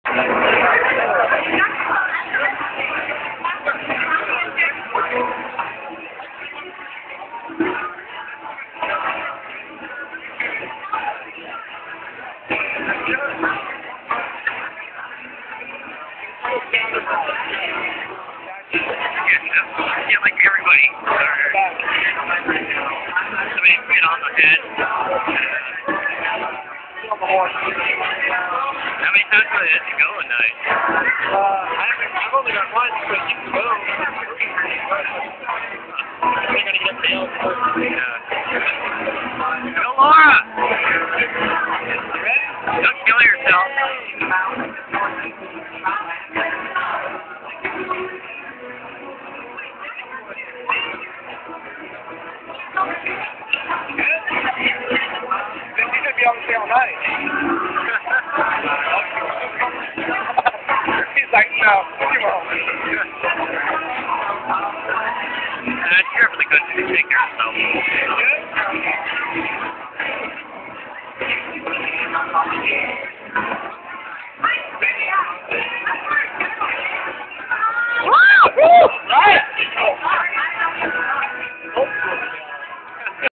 Laura riding the bull at KJs
Laura riding the bull at KJs. It was awesome. And then she fell off. We all did. You learn to get past these things.